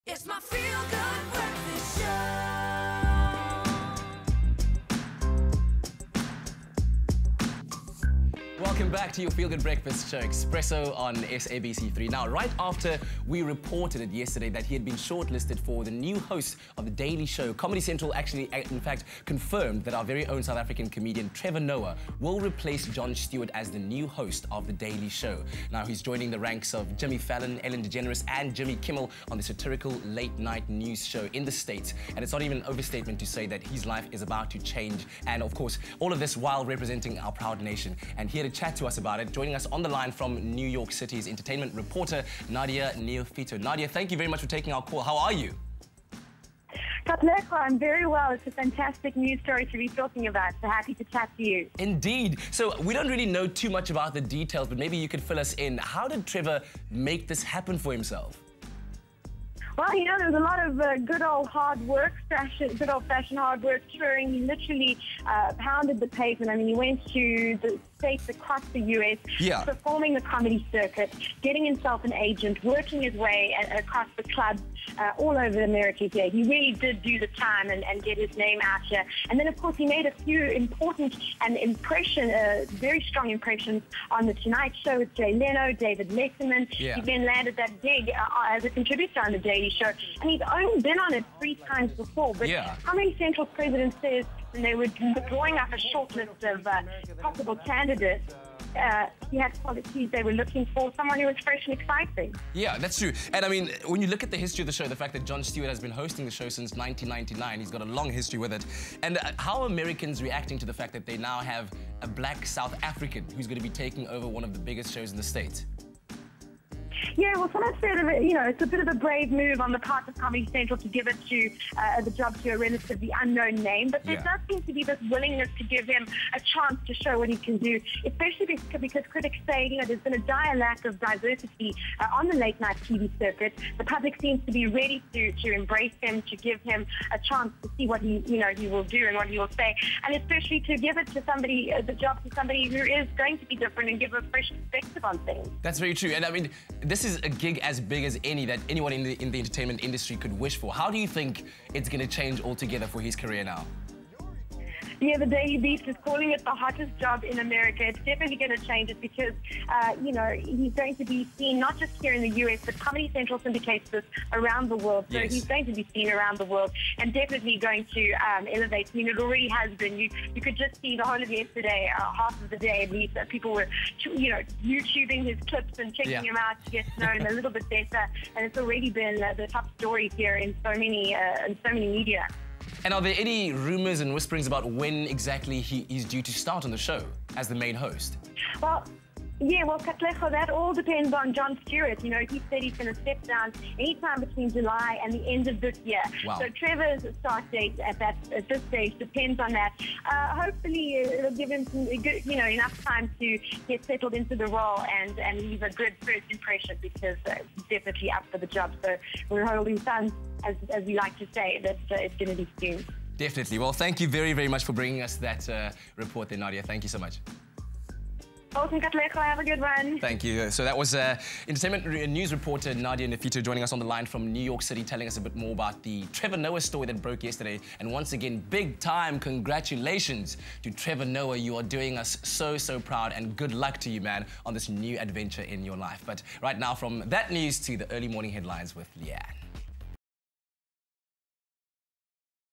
Trevor Noah to anchor The Daily Show
Live phone call with SA entertainment reporter Nadia Neophytou about Trevor Noah’s new gig s the host of the daily show.